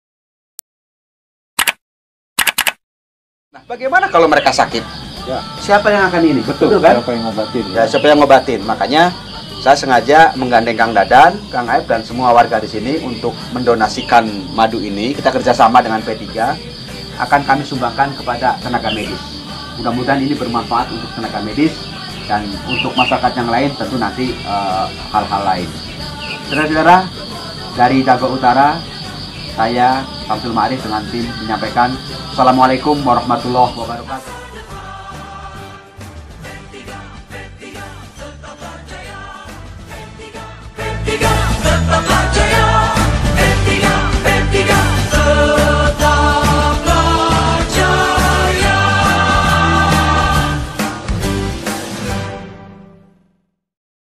Konsumsi Madu Untuk Melawan Corona Ep. 05
Episode 05. Video akhir Perjalanan Kami sebagai Team
semoga video ini bermanfaat, dan menjadi spirit bagi kita semua dalam memghadapi Pandemi Corona di Jawa Barat. Salam Sehat bagi kita semua. Wass...